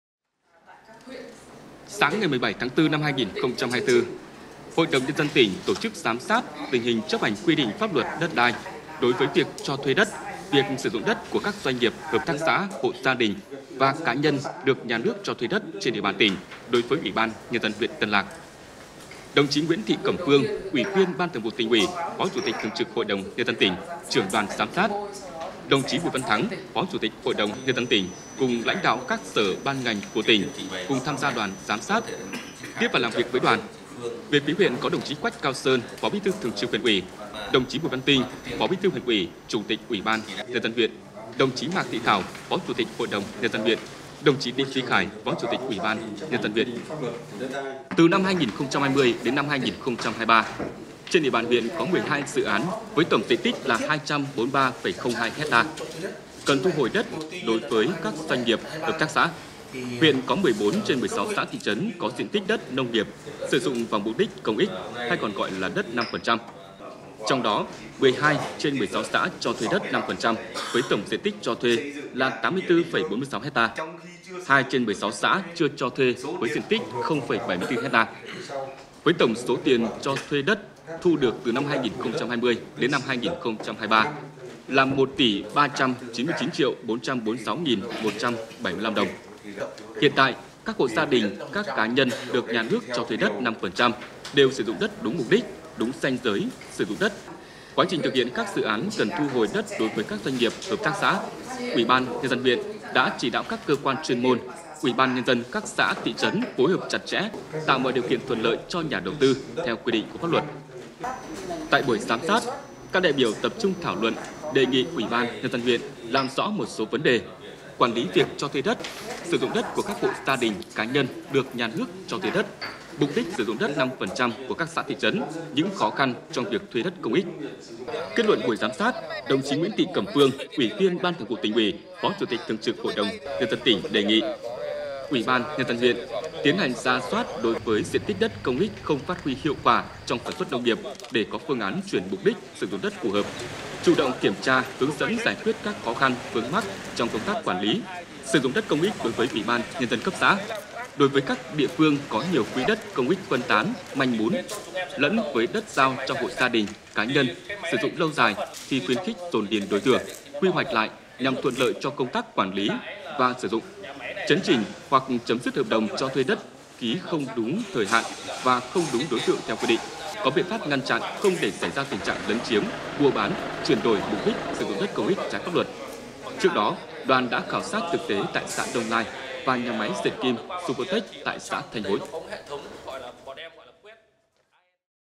HĐND tỉnh tổ chức giám sát tình hình chấp hành quy định pháp luật đất đai đối với UBND huyện TL
Hội đồng nhân dân tỉnh tổ chức giám sát tình hình chấp hành quy định pháp luật đất đai đối với Ủy ban nhân dân huyện Tân Lạc.